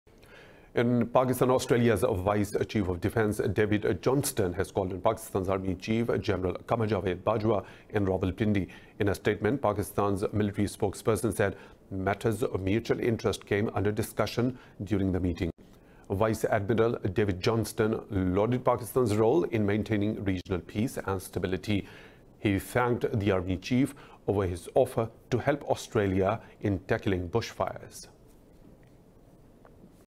Pakistan's Army Chief Offers Australia Help In Tackling Bushfires | Indus News
Australia’s Vice Chief of Defence David Johnston has called on Pakistan’s Army Chief General QamarJavedBajwa in Rawalpindi. He thanked the army chief over his offer to help Australia in tackling bushfires.

Watch Indus News Live: youtu.be/SuicpT5sZvI